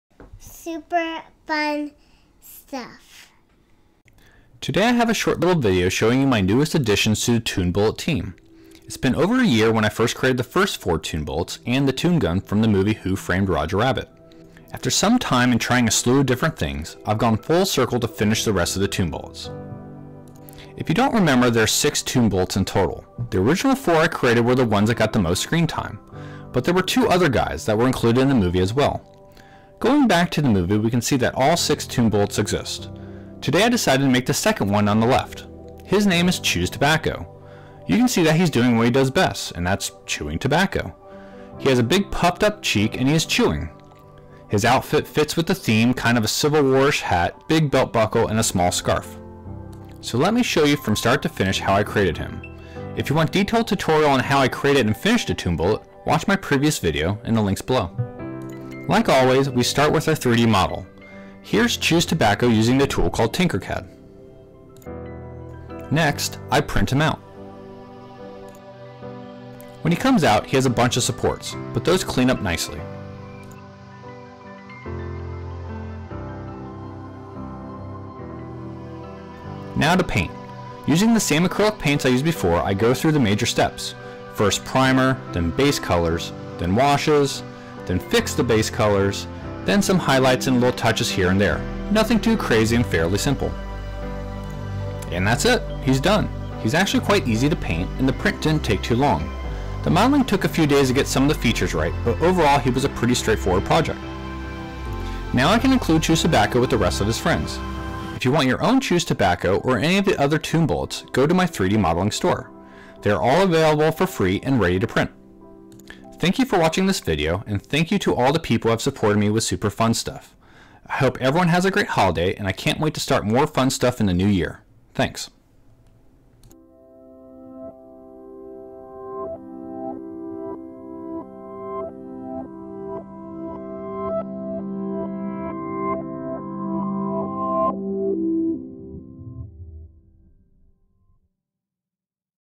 Chewing Tobacco - New Toon Bullet Addition
Let's welcome our newest toon bullet, name Chews Tobacco. This toon bullet has a big wad of tobacco in his mouth, and is sporting a nice chin strap beard and civil war looking hat. He's a great addition to the toon bullet team. Now I have 5 out of 6 toon bullets complete, with only one guy left.

Chews Tobacco is available for free on my 3D model store